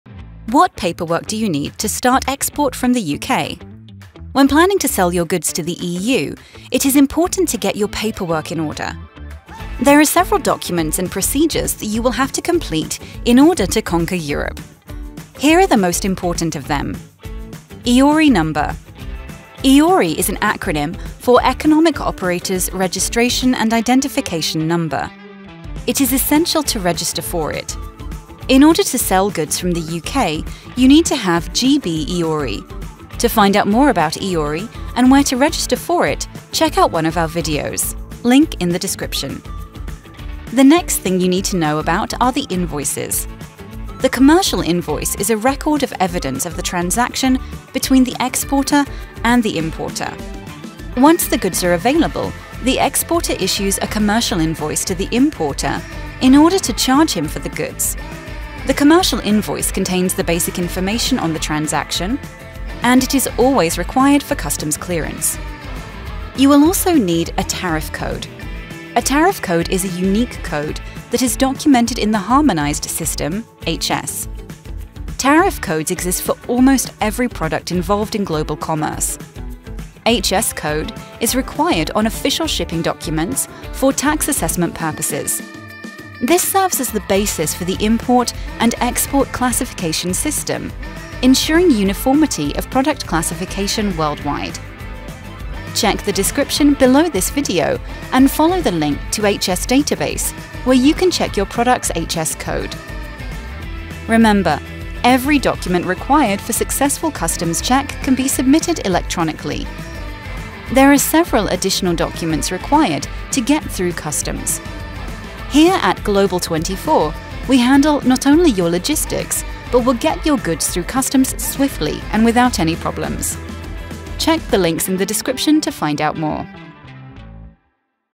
What paperwork do I need to start export from UK | GLOBAL24
When planning to sell your goods to the EU, it is important to get your paperwork in order. There are several documents and procedures that you will have to complete in order to conquer Europe. Check out the most important of them: EORI number, The Invoice, Tariff code (HS code).